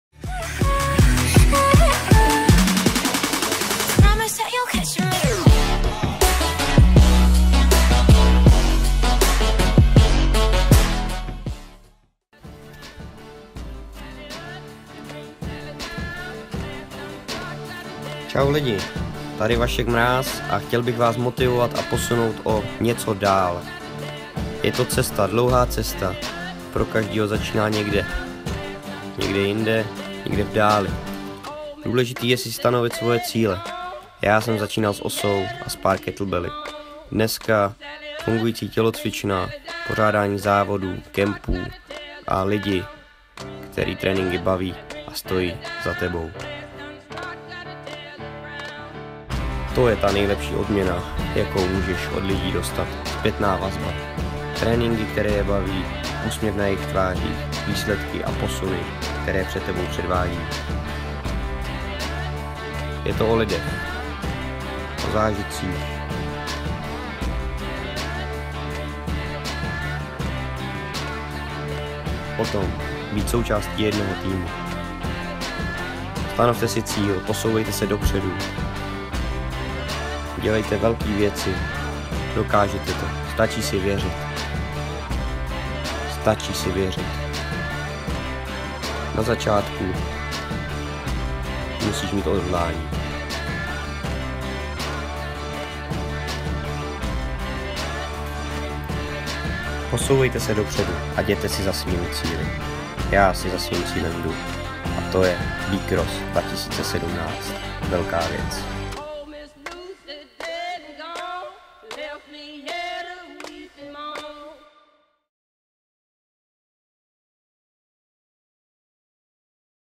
Motivace, trénink, já & pár slov BCROSS
Video z léta 2016, které se dostává ven až nyní :) Pár slov ode mě.
Zvládnete vše, jen si zatím musíte jít!

Já mám letos velké plány, cíle, které si chci splnit a udělám proto vše.
Bavte se tréninkem a dělejte co Vás baví!!